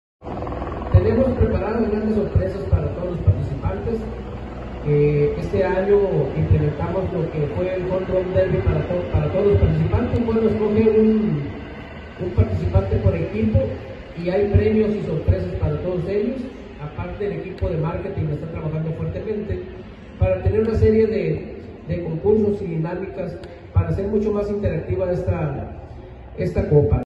Anuncian la apertura de inscripciones para el Torneo Periquitos Acapulco 2022
Anuncian la apertura de inscripciones para el Torneo Periquitos Acapulco 2022 que se realizará del 12 al 16 de octubre